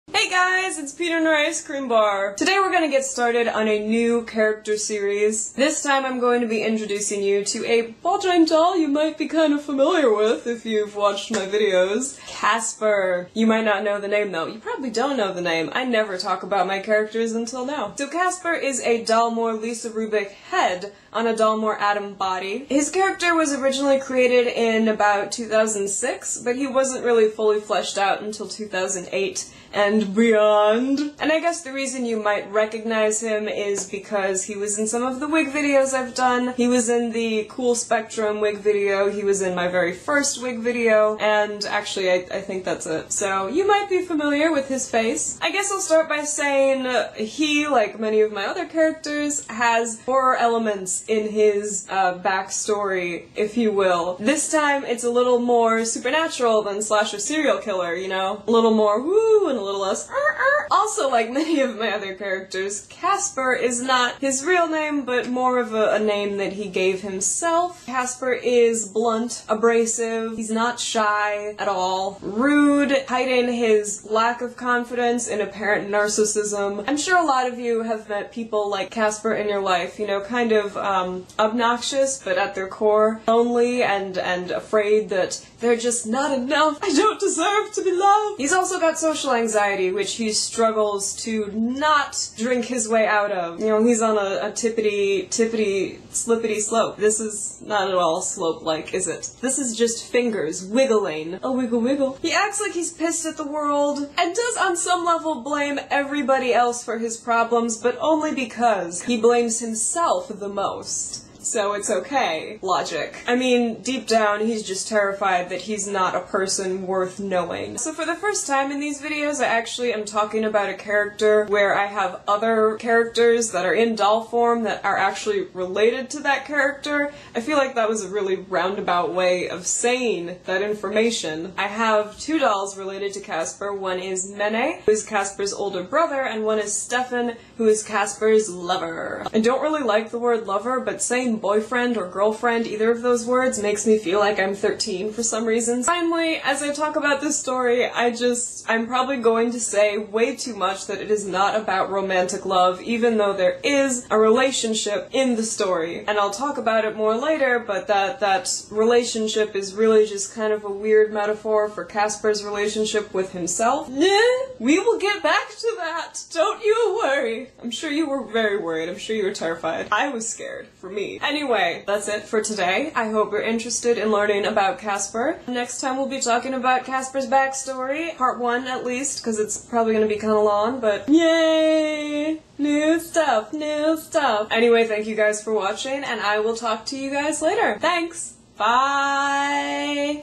Mini Character Videos :: Casper Intro
A short introduction to my character Casper, who is a Dollmore Lisa Rubik head on a Dollmore Adam body. This video should have been up last week, but I still haven't figured out my new editing software XD

This is part of a longer ‘series’ of videos I hope to do for all my dolls about their development and progression as written characters as well as their evolution as Ball Joint Dolls/how they have been effected by being realized as BJDs.